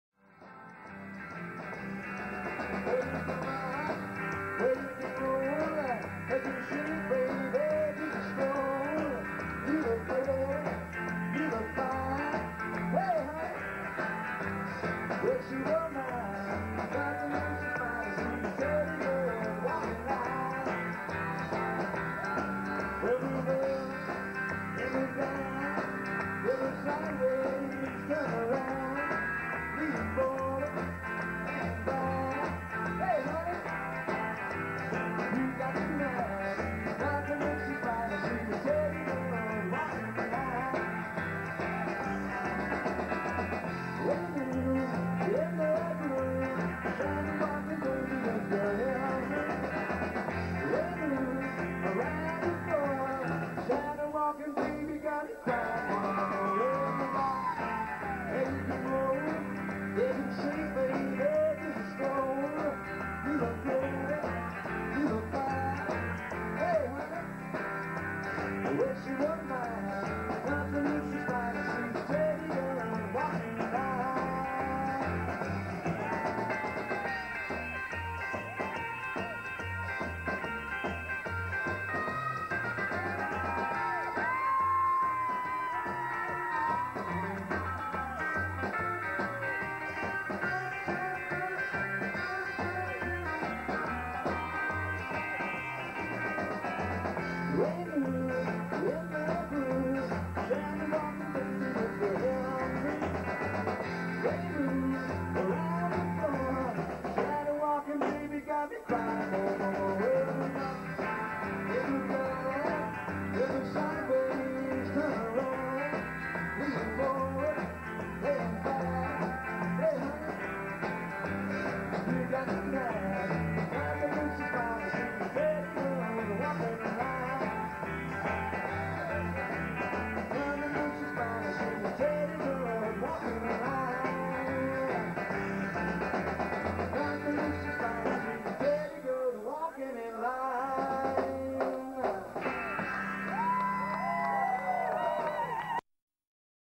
Sandy Ford
Concert gravat a Great Yarmouth en el Teddy Boy Weekenders per el Dj Eddie-Cesc en els anys 90s.